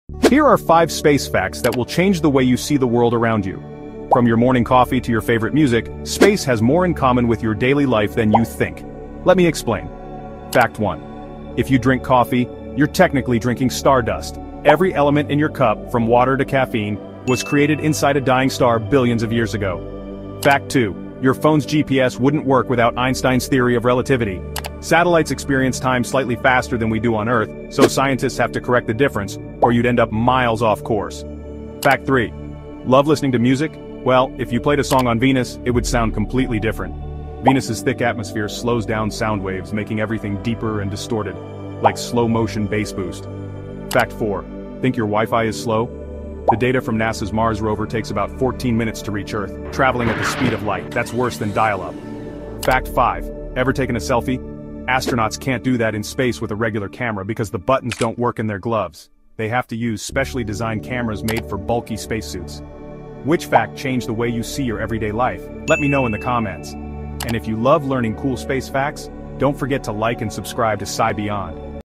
5 Space Facts That Will Blow Your Mind 🚀 (Your Coffee Is Stardust!)
Unlock 5 mind-blowing space facts hiding in your daily life! 🚀
From stardust in your morning coffee to Einstein’s relativity powering your GPS, this short video shows how deeply you’re connected to the universe. Discover how music would sound on Venus, how long NASA’s Mars rover signals take to reach Earth, and more crazy cosmic connections you’ve never thought about.